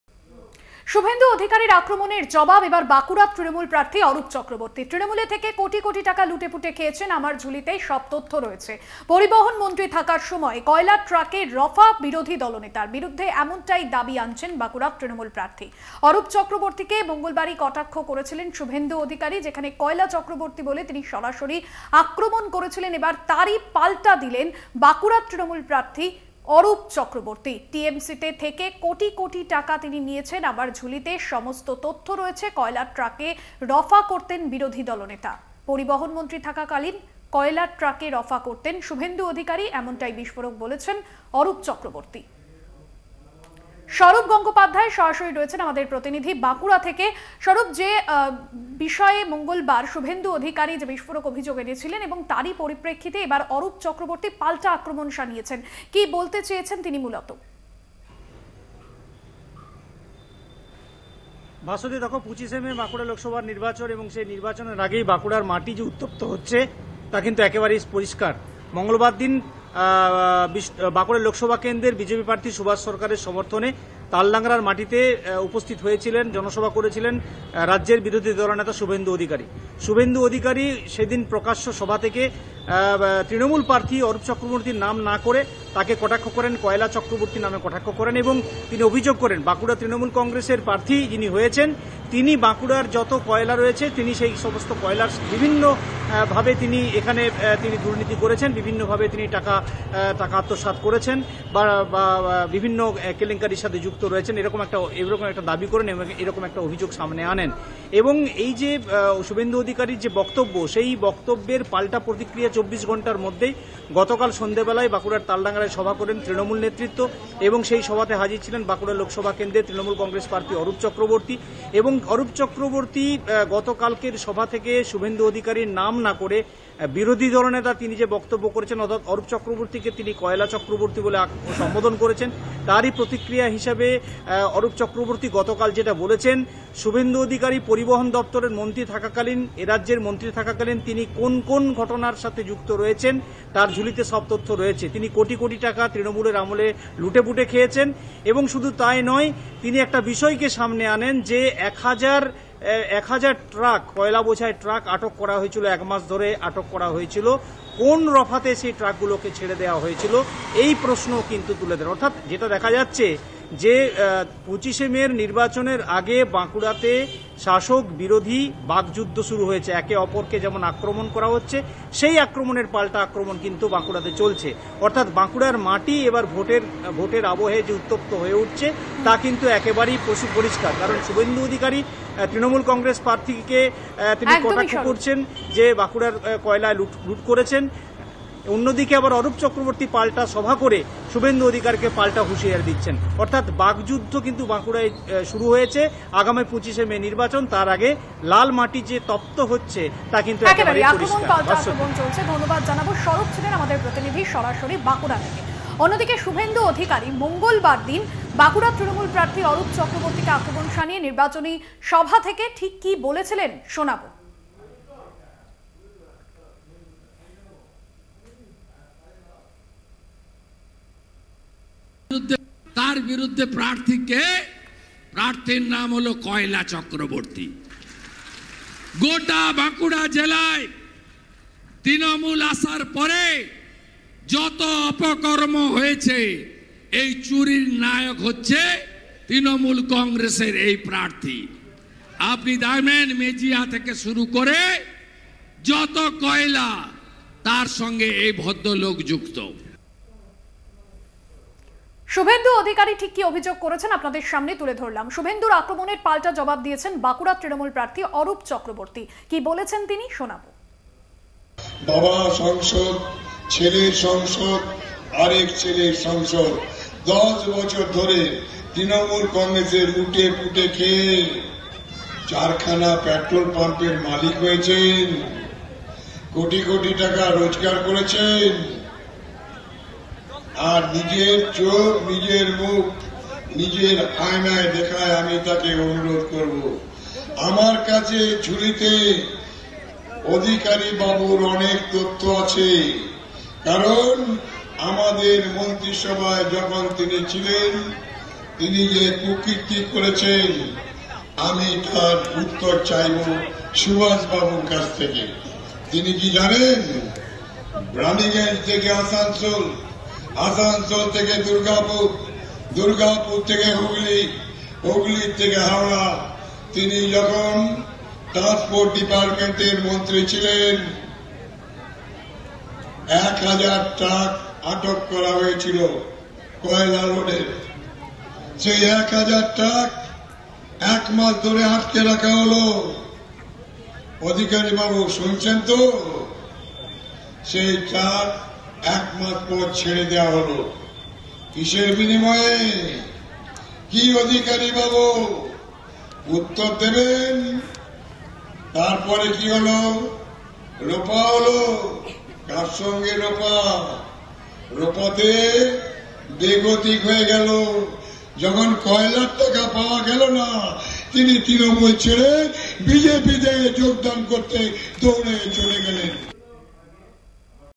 Arup Chakraborty | 'পরিবহণমন্ত্রী থাকতে কয়লার ট্রাকে রফা', শুভেন্দুর আক্রমণের জবাব অরূপ চক্রবর্তীর
'পরিবহণমন্ত্রী থাকতে কয়লার ট্রাকে রফা', শুভেন্দুর আক্রমণের জবাব অরূপ চক্রবর্তীর

website link for this copy :
@Kolkata Tv